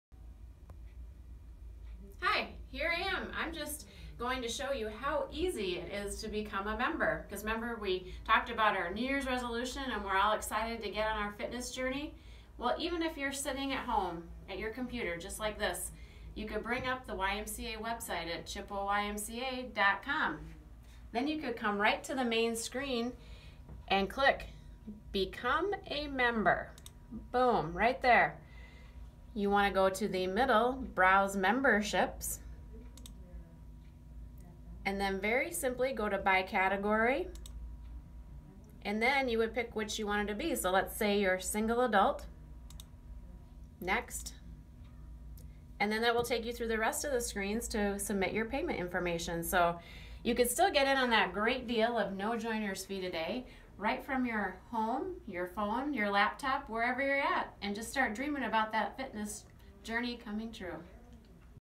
YMCA 101 : Chapter 1
Jennifer is here to show you how easy we have made it to become a member of the Chippewa Valley Family YMCA!